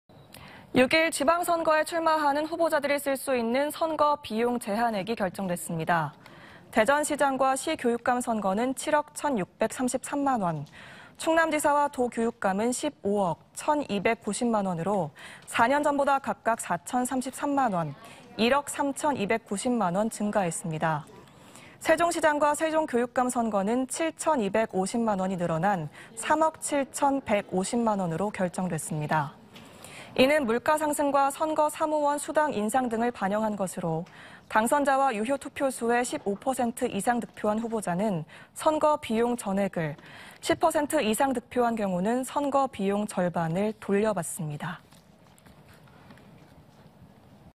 6.1 지선 선거비용제한액 결정..4년 전보다 늘어｜ TJB 대전·세종·충남뉴스
6.1 지방선거에
출마하는
후보자들이
쓸 수 있는
선거비용
제한액이 결정됐습니다.

대전시장과 시교육감 선거는 7억 천 6백 33만원, 충남지사와 도교육감은 15억 천 2백 90만원으로 4년 전보다 각각 4천 33만원, 1억 3천 290만원 증가했습니다.

세종시장과 세종교육감 선거는 7천 2백 50만원이 늘어난 3억 7천 백 50만원으로 결정됐습니다.

이는 물가 상승과 선거사무원 수당 인상 등을 반영한 것으로 당선자와 유효투표수의 15% 이상 득표한 후보자는 선거비용 전액을, 10% 이상 득표한 경우는 선거비용 절반을 돌려받습니다.